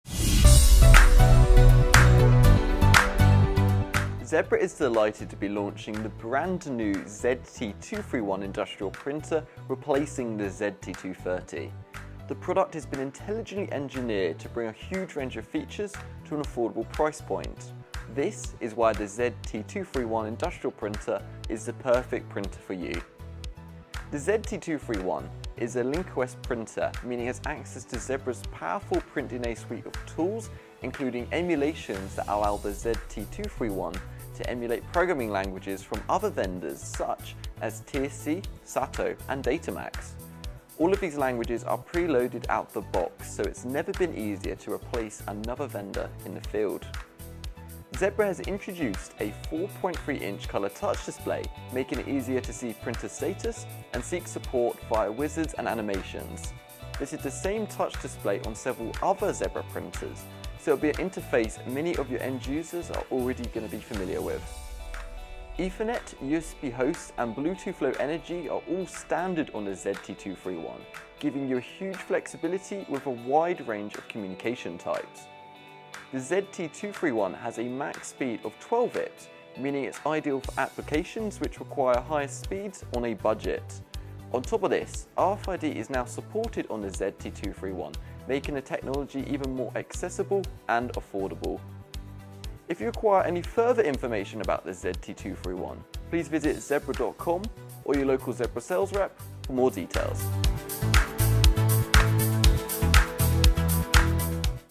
Introducing the ZT231 Industrial Printer | Zebra
Zebra is delighted to be launching the brand new ZT231 Industrial Printer, replacing the ZT230. The product has been intelligently engineering to bring a huge range of features at an affordable price point. Zebra EMEA Regional Product Manager, Alex Holdsworth, provides a quick look at Zebra’s latest value tier industrial printer.